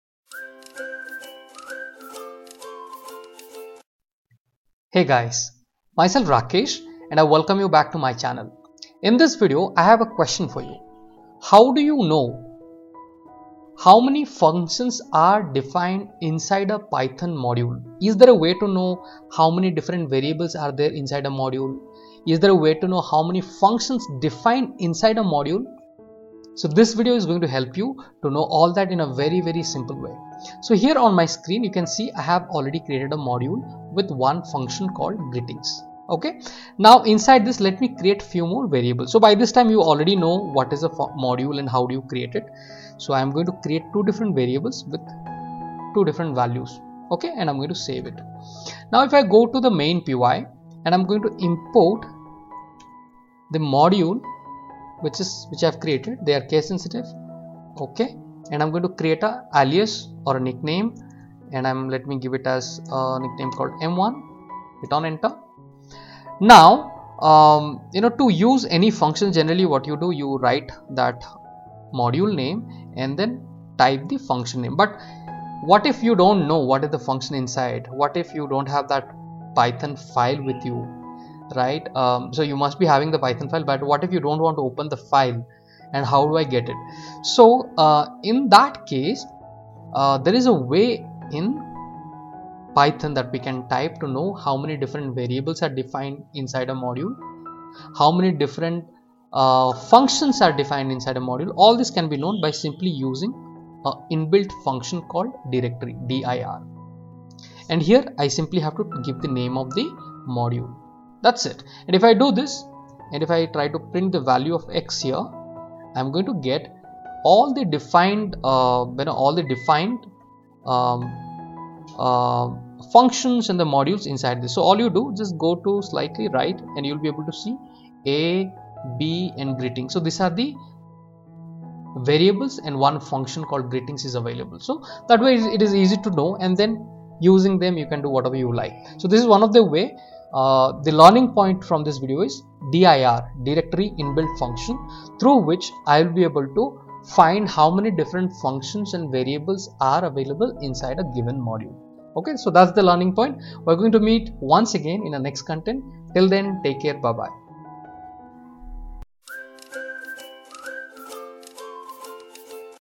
#68 How To see all the functions available in a Python module | How To list functions in Python
The dir() function returns all of the supplied object's properties and methods, but not their associated values. Even built-in attributes that are the default for all objects will be returned by this function. To list all functions in a Python module you can use dir(module). Watch this YouTube video to clearly understand the dir function in Python to list all variables and functions in Python module.